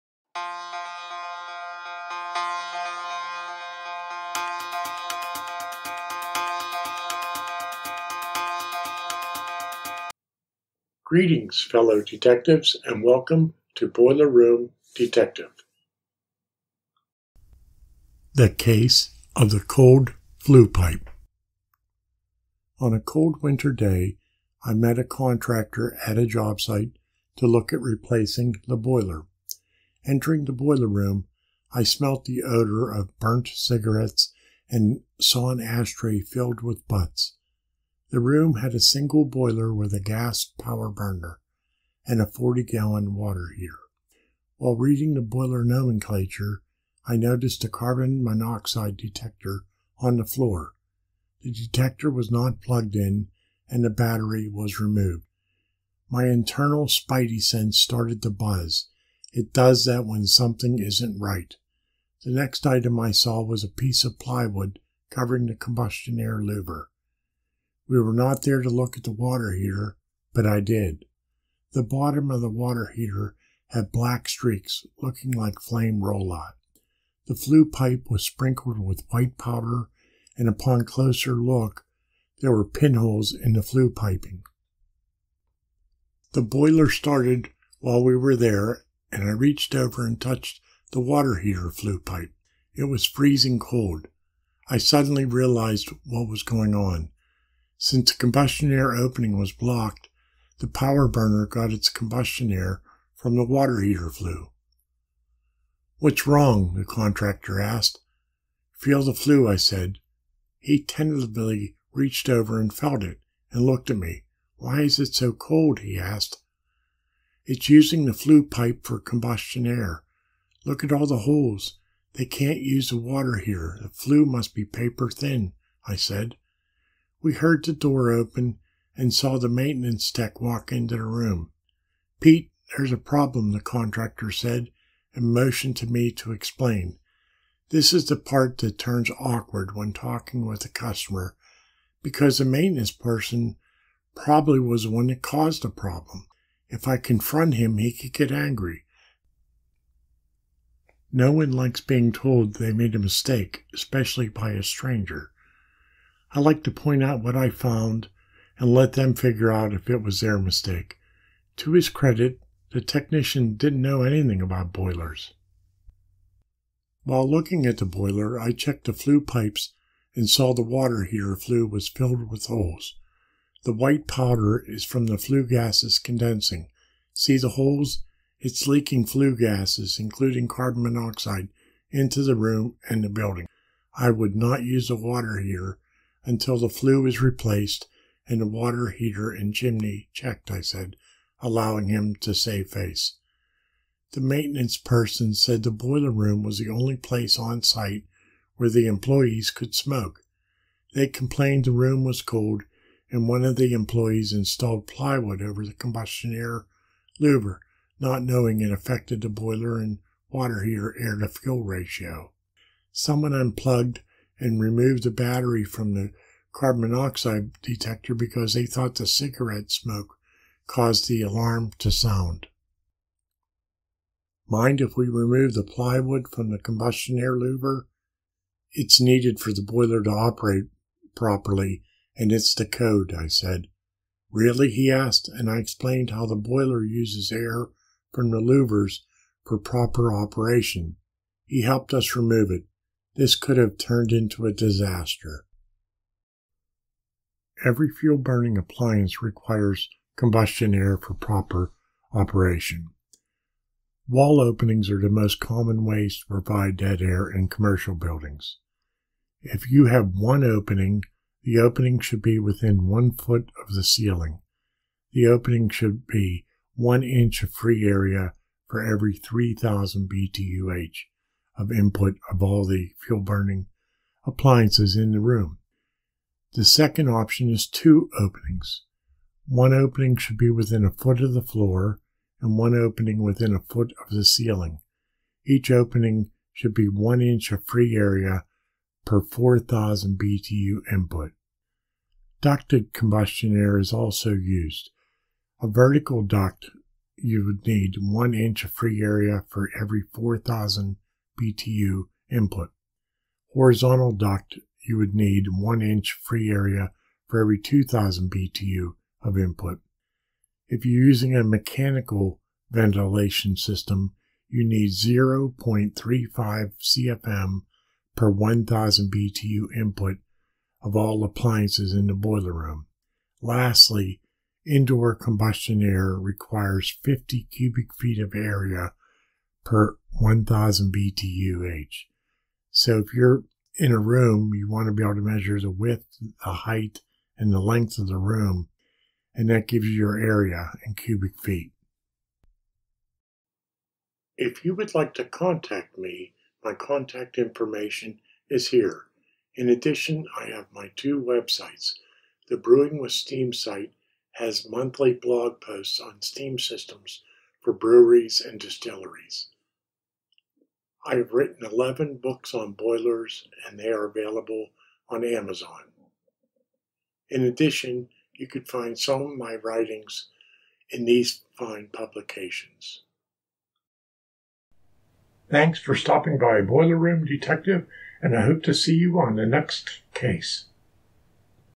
The case of the Cold Flue Pipe
In this case, I find a boiler room where the combustion air louver is blocked with plywood. The boiler was able to find the combustion air it needed in a strange place. At the end of the video, I show the combustion air required for a boiler according to the International Fuel Gas Code